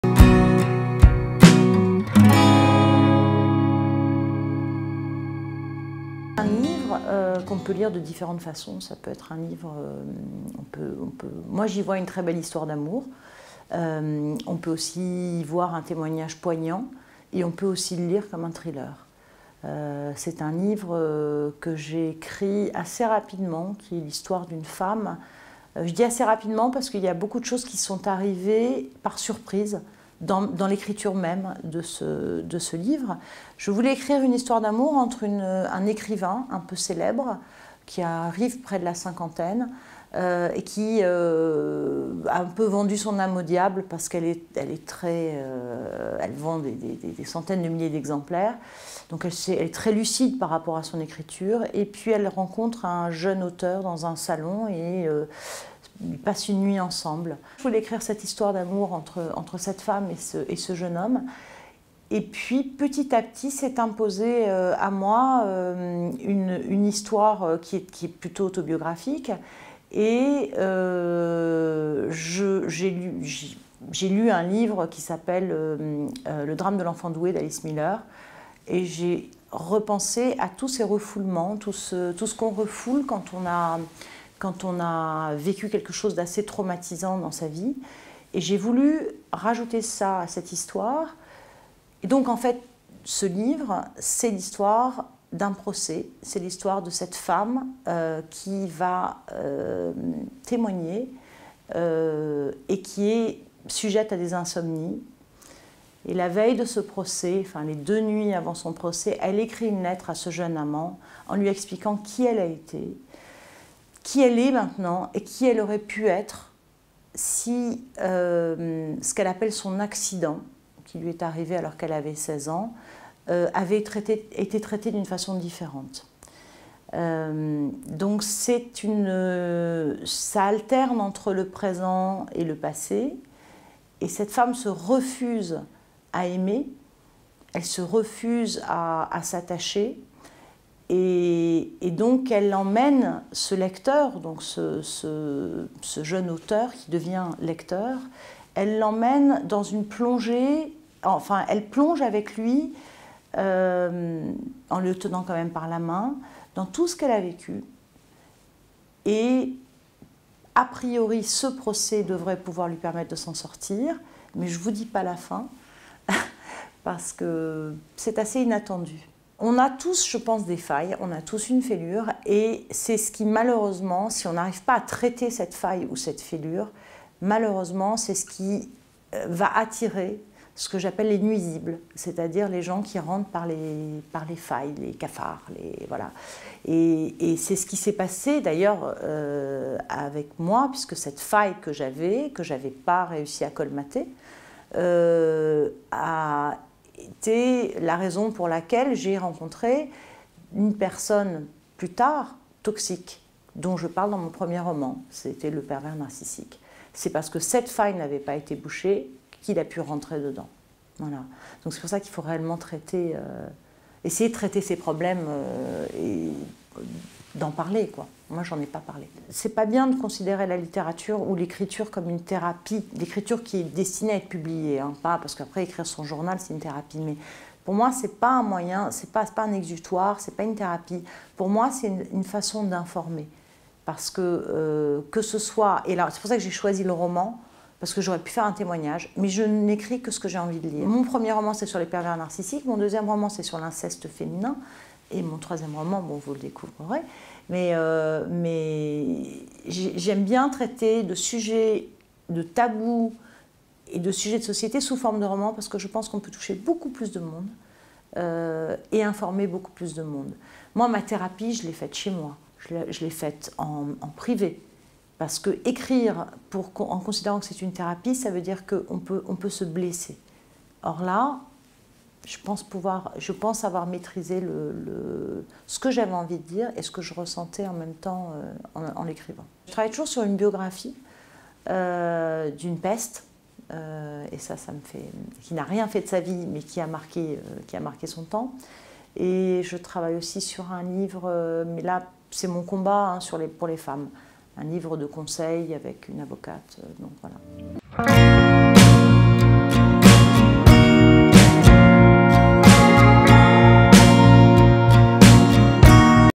Sylvie Le Bihan - Qu'il emporte mon secret
Sylvie Le Bihan vous présente son ouvrage "Qu'il emporte mon secret" aux éditions Seuil. Rentrée littéraire d'hiver 2017.